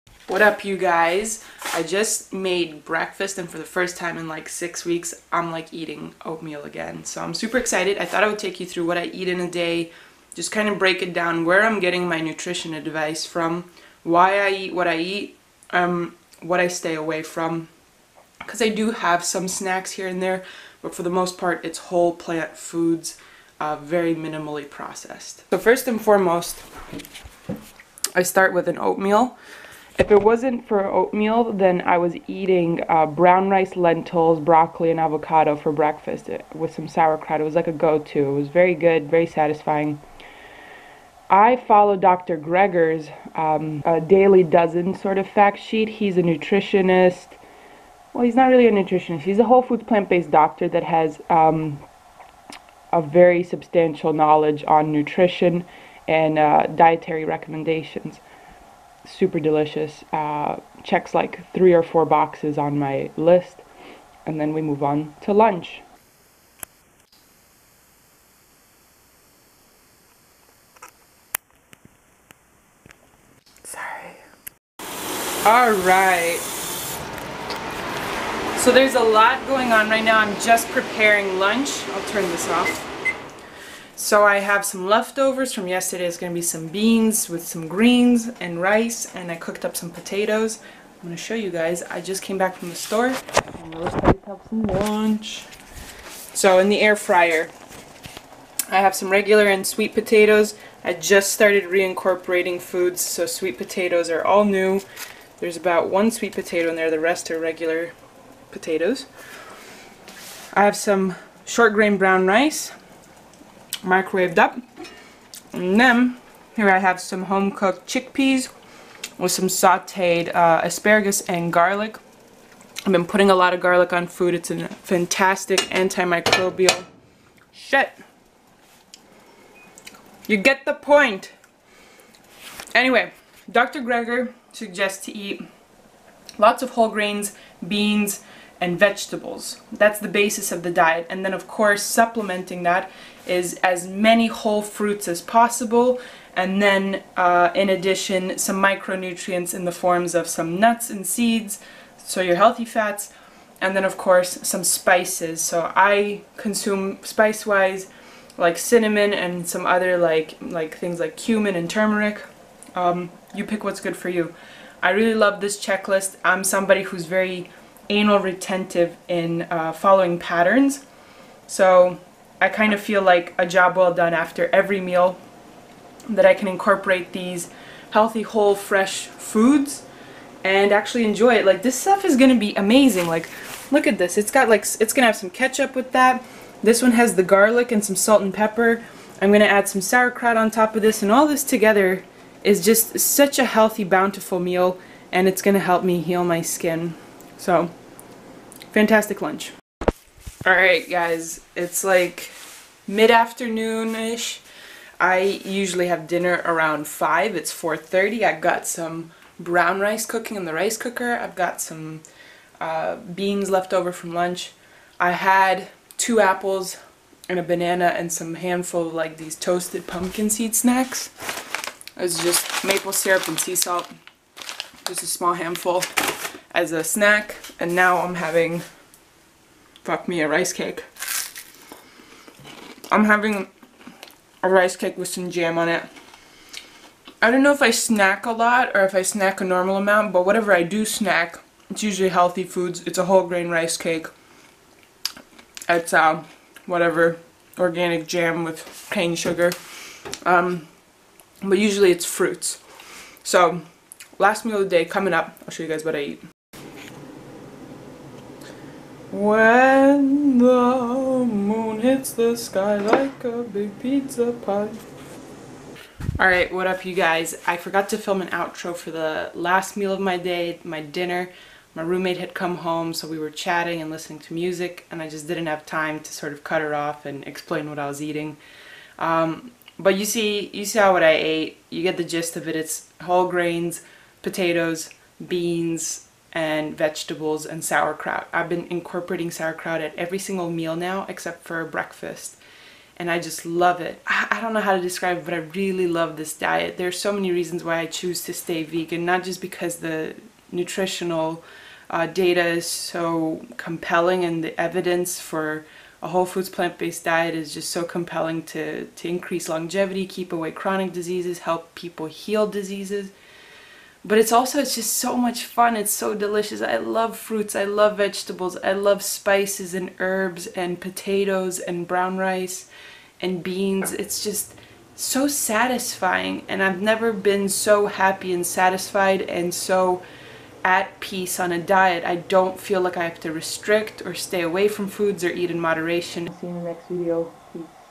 My Favorite Foods for Eczema
Hi, I'm Susan, I've had eczema most my life. After following a strict, whole foods plant-based diet for 8 months, I put my eczema into remission thanks to the help of plant-based doctors like Dr. John McDougall.

After six months of remission, I took some medications to save my sick ass and ended up damaging my immune system/gut health, sending me into a massive flare up that lasted 9 months.

Now, I'm applying my medical, gut health test results to inform my eczema treatment plan to make this go away for good. Turns out, when you know what your problem is, it becomes a lot easier to fix.